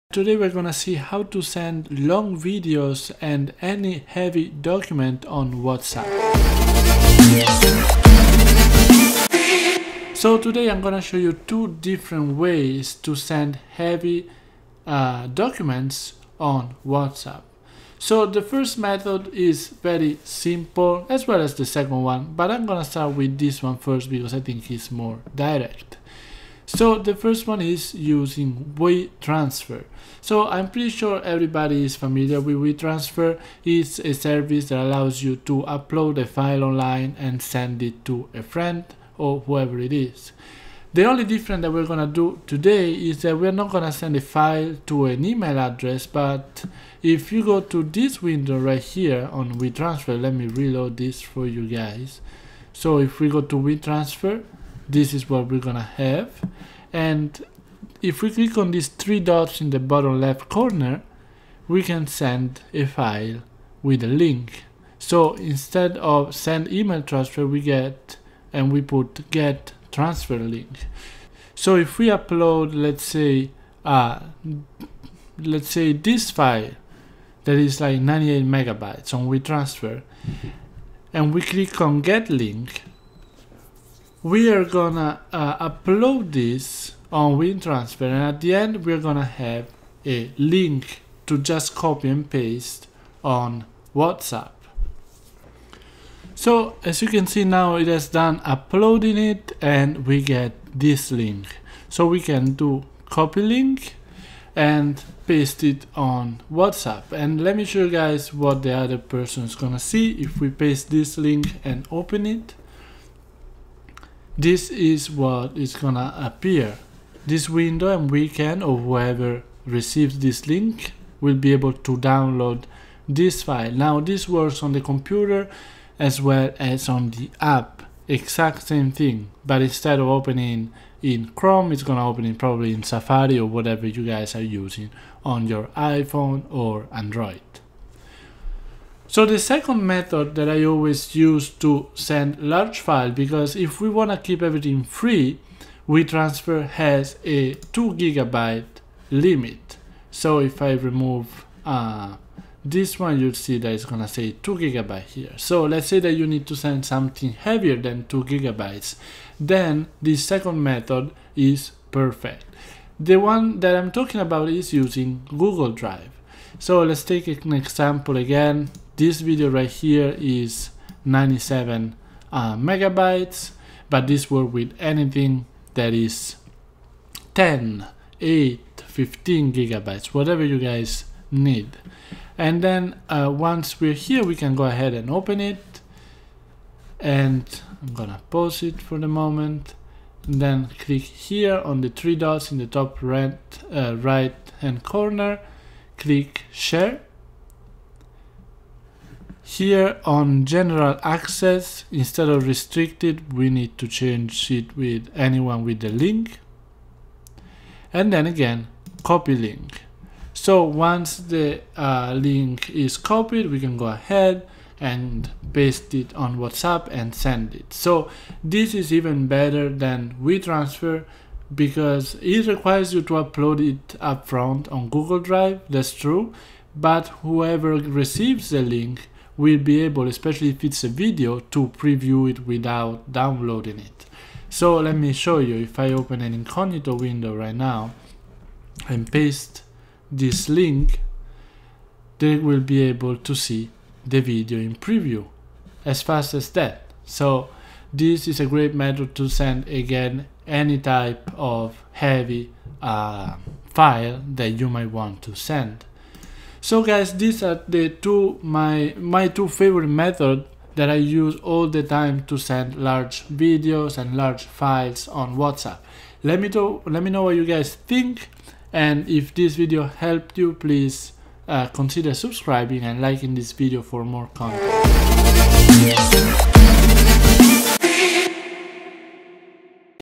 How to send long video on WhatsApp | Send files up to 10GB for FREE
WhatsApp is a free messaging app for smartphones that lets you send text messages, photos, and videos. But did you know you can also send long videos on WhatsApp? Watch this video to learn how!